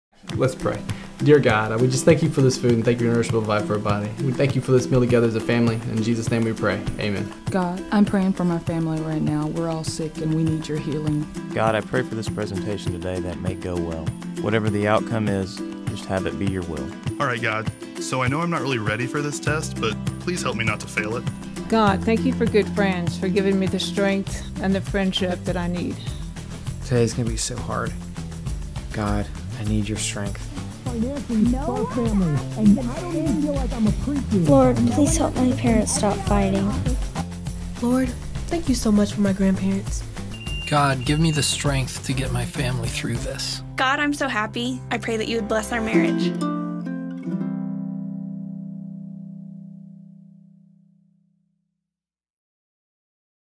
Breakthrough Prayer | New teaching series at Free Grace United
People say prayer changes things, but have you ever experienced it for yourself? Is there actually power in prayer, or is that just a cliche?

For three weeks, we are going to focus on 3 breakthrough prayers found in Scripture. Come learn why inviting the supernatural into your everyday life can transform everything and figure out how to start!

BreakthroughPrayer starts July 13 & 14.